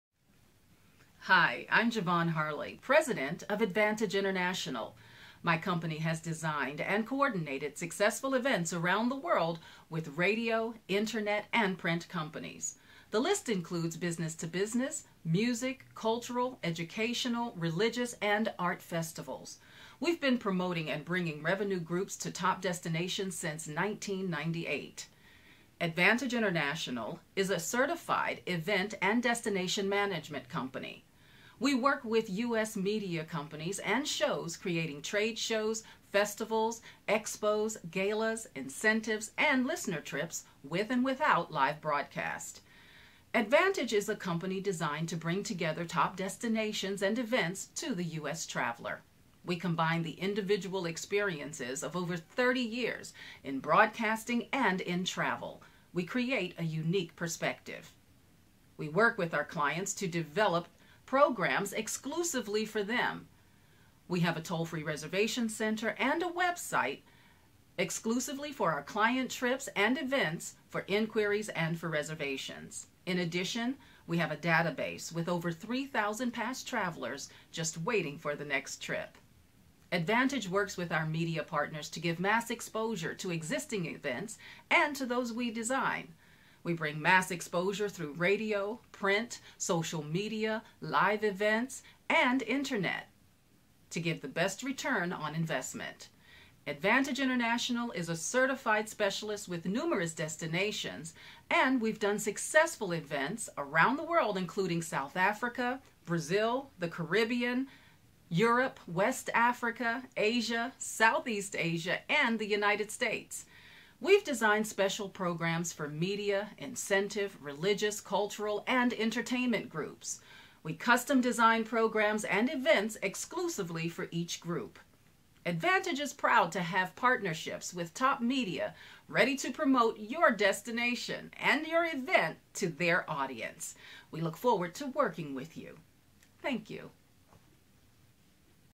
Advantage International profile
Ja'Vonne Harley's introduction of Advantage International, LLC